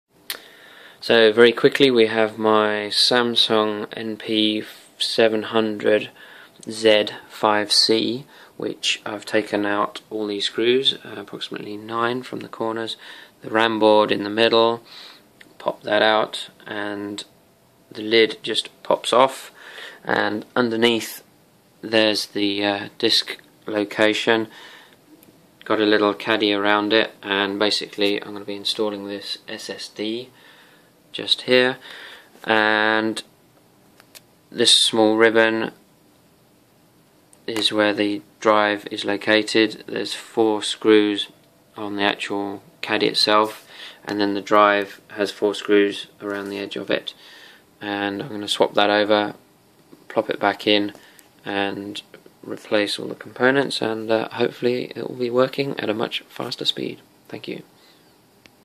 Samsung series 7 NP700Z5C ssd disk swap
A very brief overview of taking apart my Samsung laptop to install an SSD in replacement of the original 2.5" 5400 rpm SATA drive - very slow.